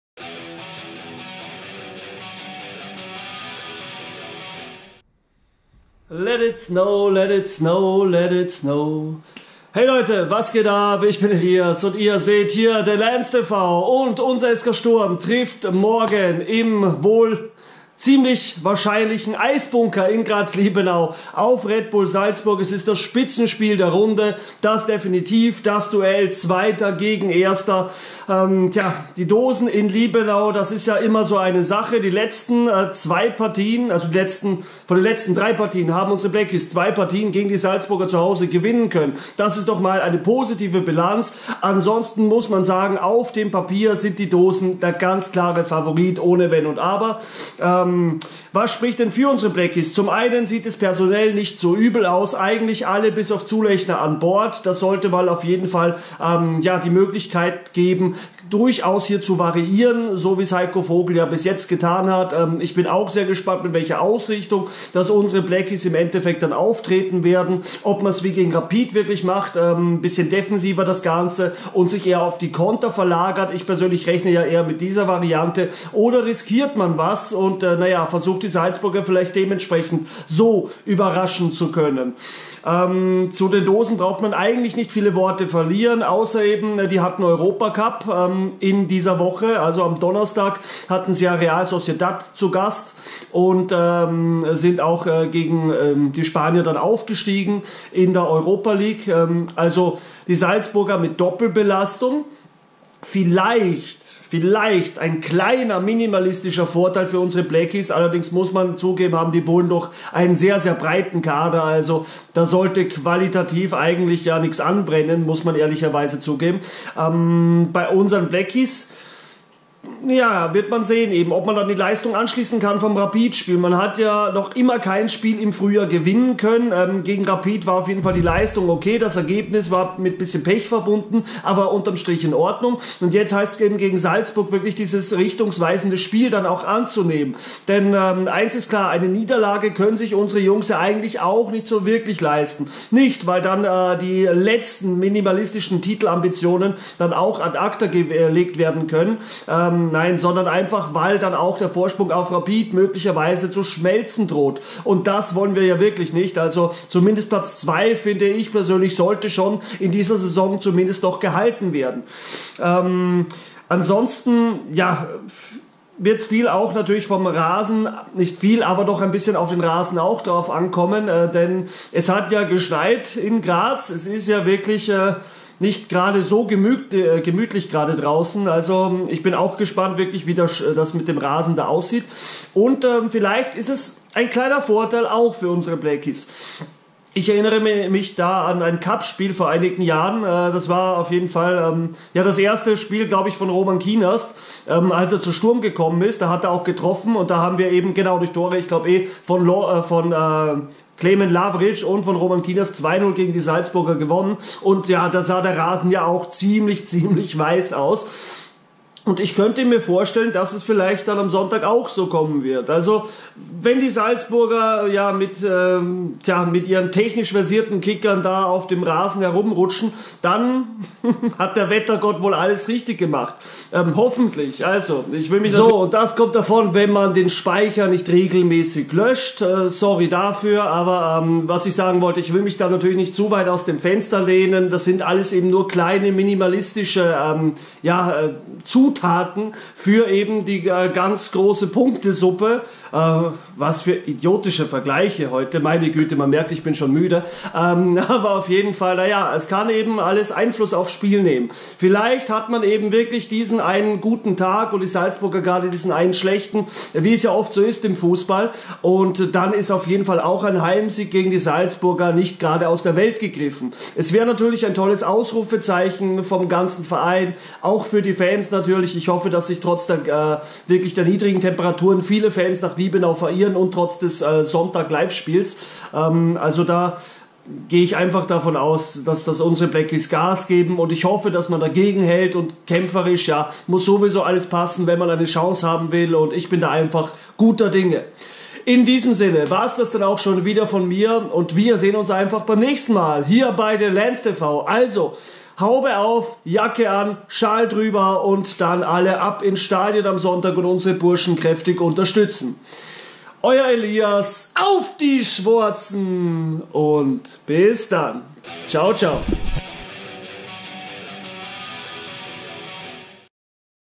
Schneespiel? SK Sturm Graz - RB Salzburg Vorbericht Tipico Bundesliga 24.Spieltag
Frau Holle ist außer Rand und Band und Graz ist von Schnee bedeckt, mal sehen was ob das Wetter unser Blackies beim unternehmen, Salzburg die erste Pflichtspielniederlage seit Sommer ( in Graz! ) zu bescheren!